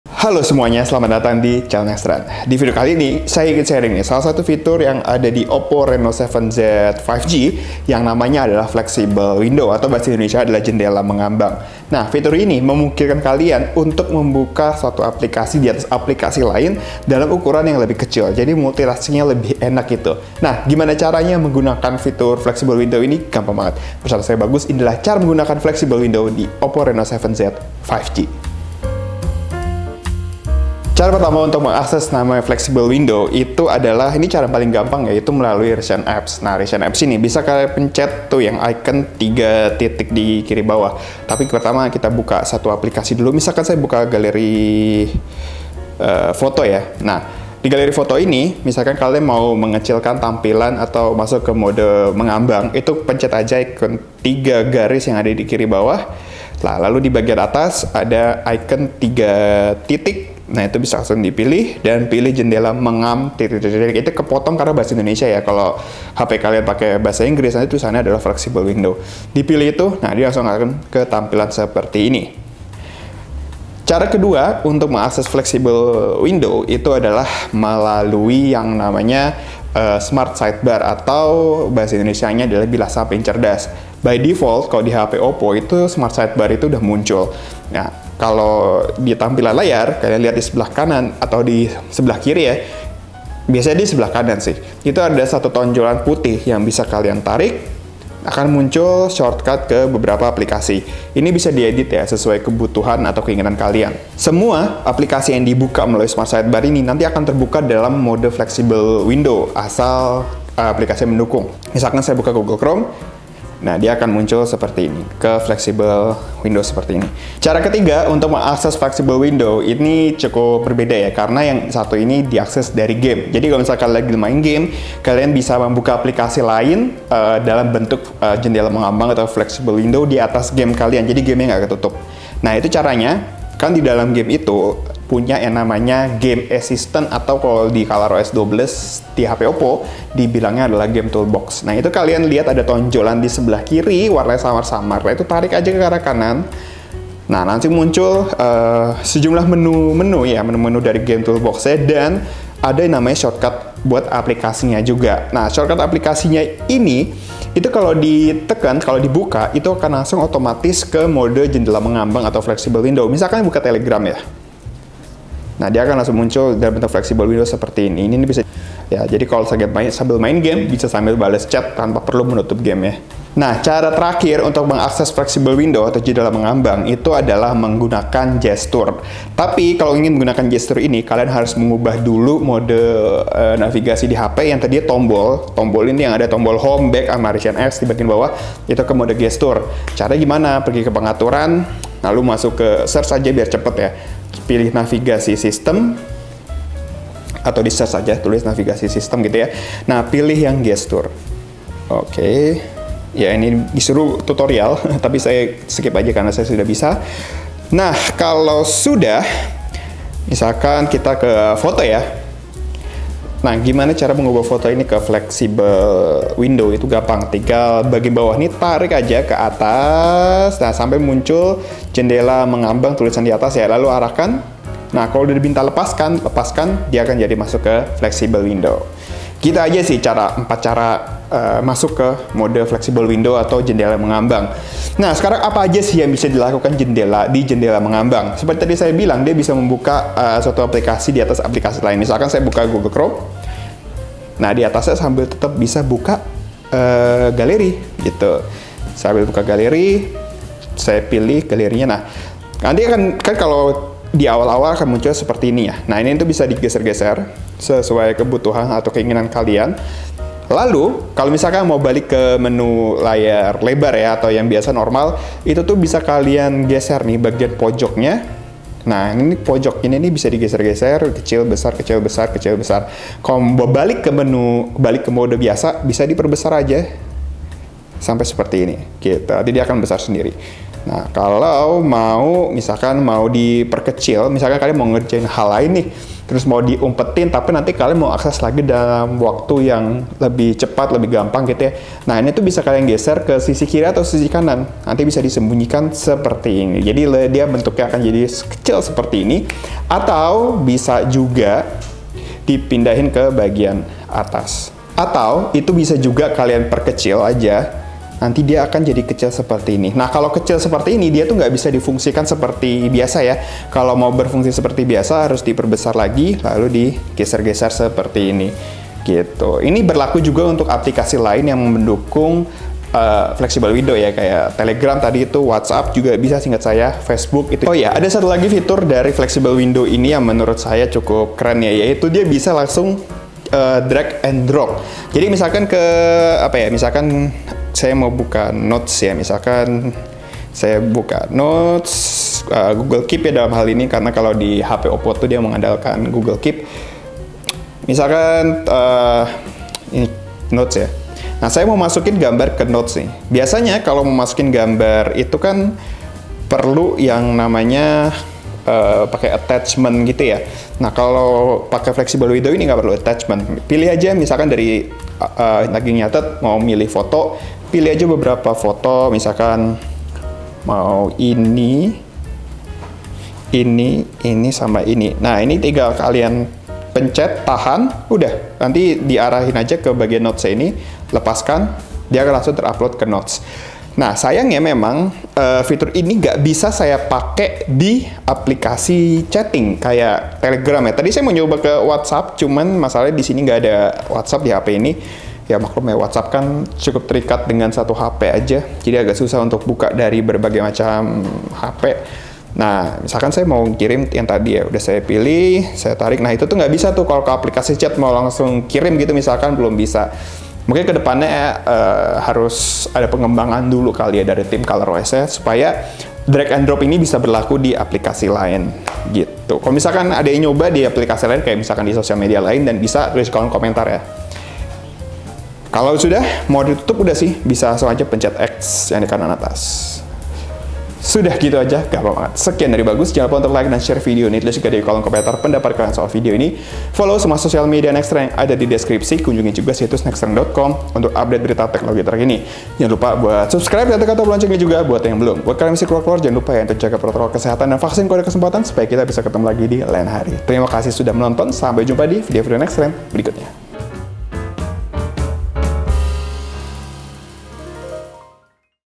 Cara Menggunakan Fitur Jendela Mengambang / Flexible Window di OPPO Reno7 Z 5G | NextTutorial
nextren.com - Untuk mendukung skenario pemakaian multitasking, OPPO Reno7 Z 5G dengan ColorOS 12 dilengkapi dengan fitur Jendela Mengambang atau floating window.

Fitur floating window dapat memaksimalkan skenario multitasking membuka lebih dari 1 aplikasi dalam sekali waktu di OPPO Reno7 Z 5G.

Cara untuk memakai floating window juga cukup beragam untuk mematikan pengguna mudah untuk menggunakan fitur ini.

00:00  Intro
00:33 Cara Mengakses Floating Window
03:55 Bisa apa saja ?
05:30 Drag n Drop